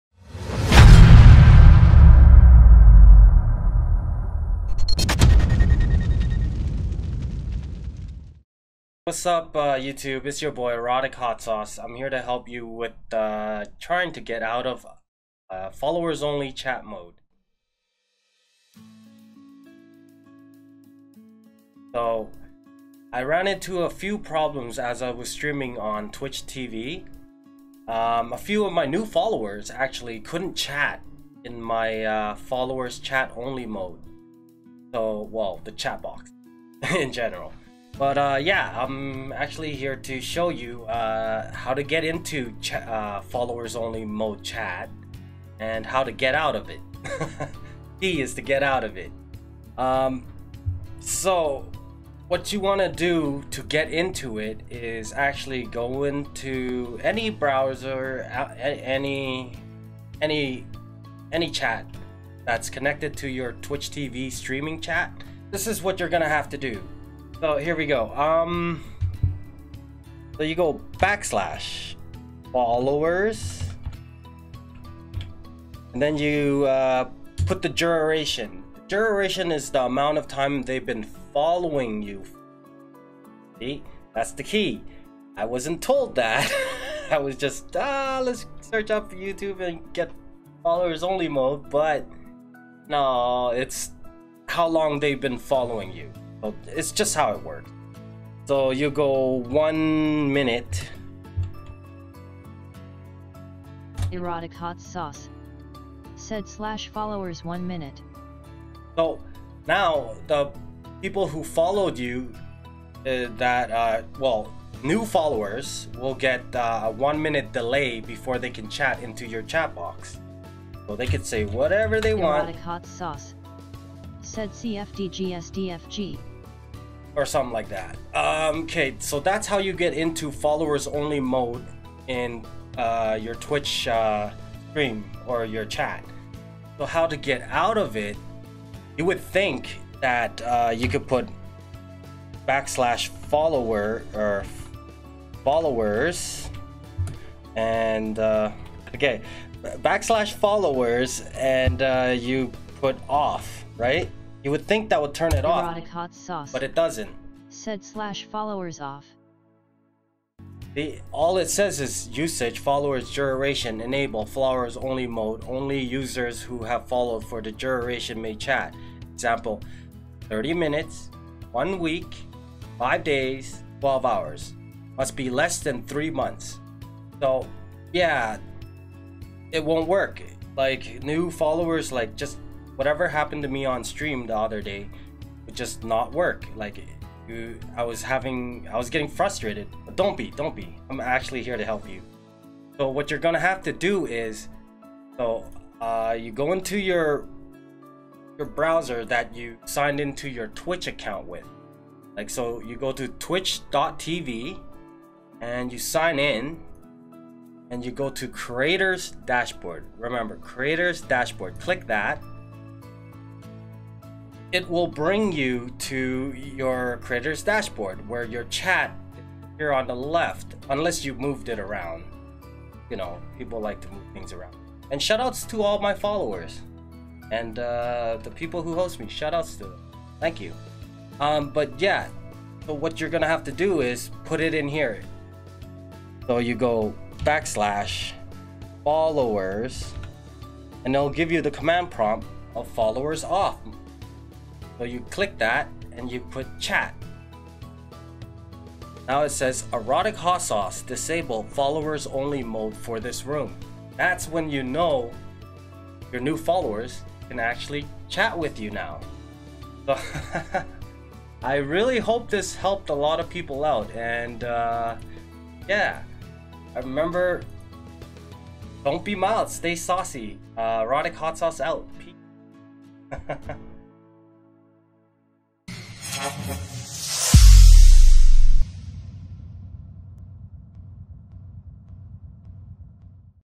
How to remove followers only mode on Twitch chat
This information is only for content creator and streamers on twitch!

Hello I'm a new twitch streamer who is trying to help the community, threw my trails/errors and finding a solution that I can help share :)

How to enable and disable twitch followers chat only mode.
You can enable it on any device that is connected to your twitch chat.
You can not disable on any devices, prior to this video at the moment.
The only way is to use your computer browser google chrome and ect...

Huge thanks to my followers and supporters!
Don't be mild, Stay Saucy!